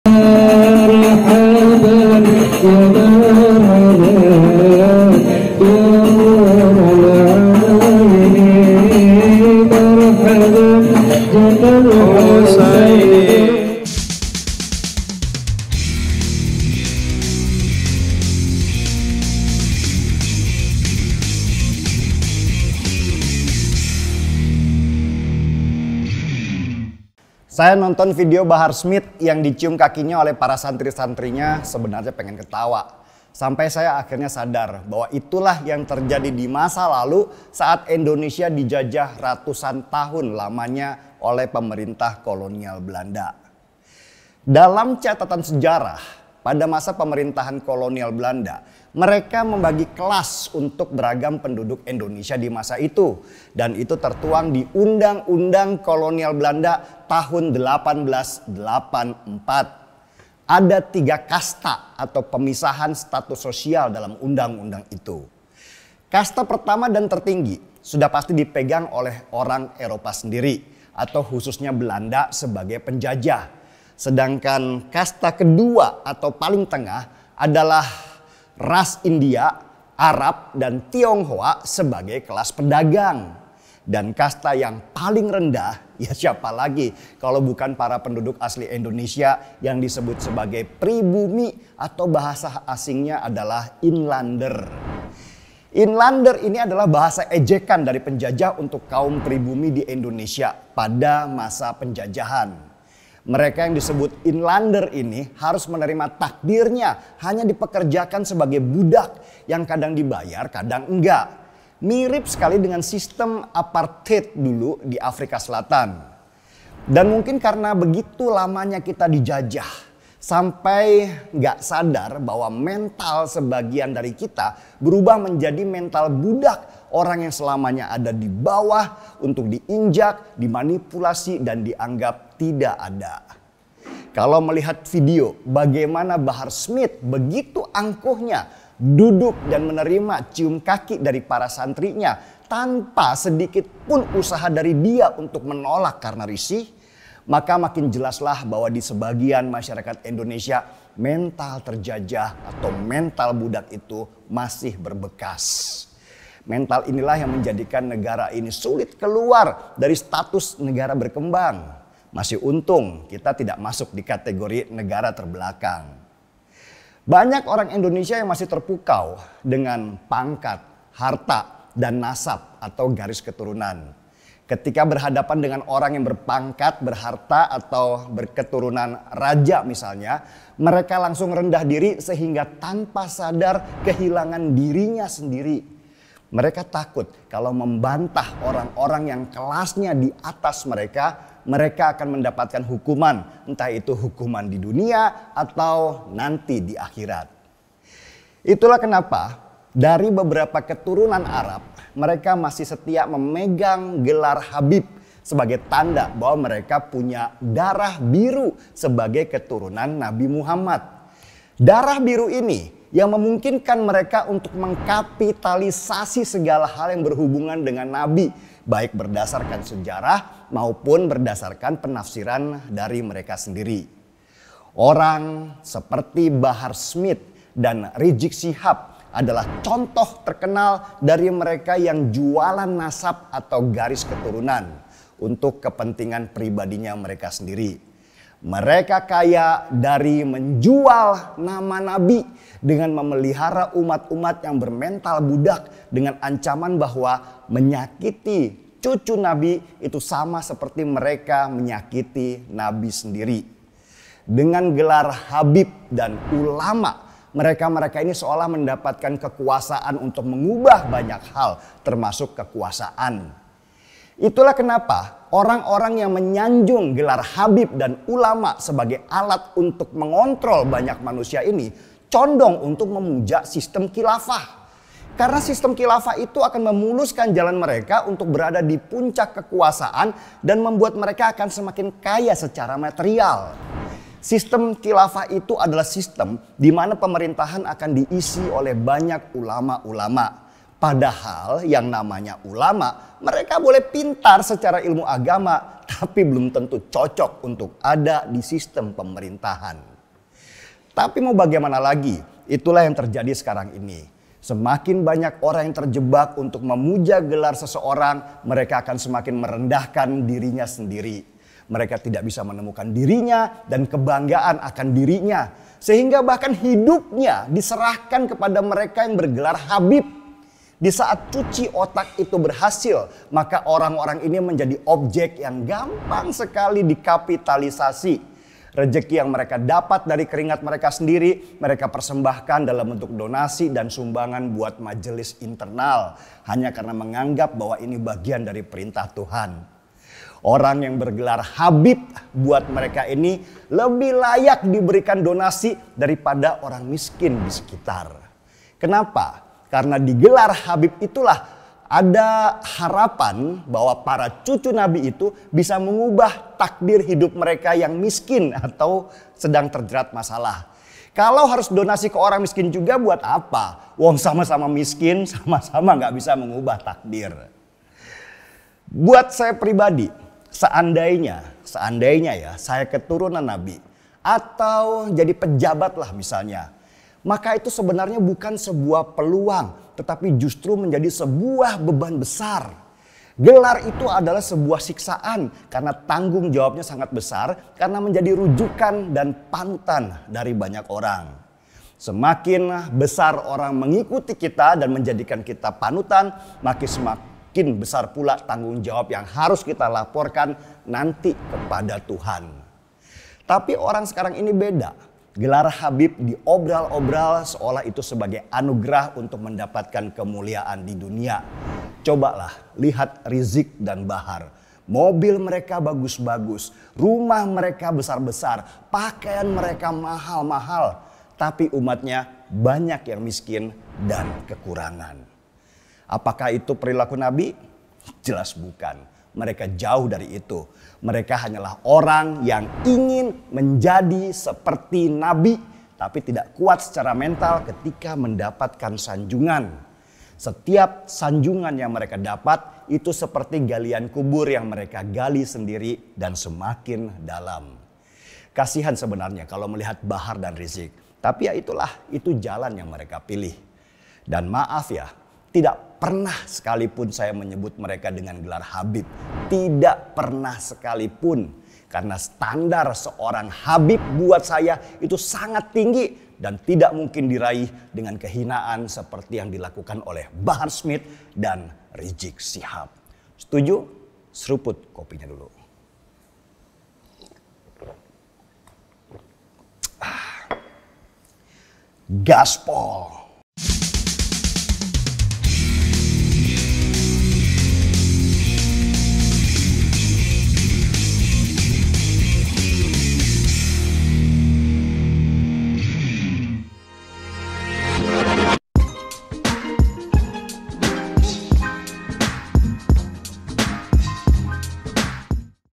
Denny Siregar: BAHAR DAN RIZIEQ GAK PANTAS JADI HABIB (Gaspol #98)
Bahar dan Rizieq mestinya malu menyandang gelar habib.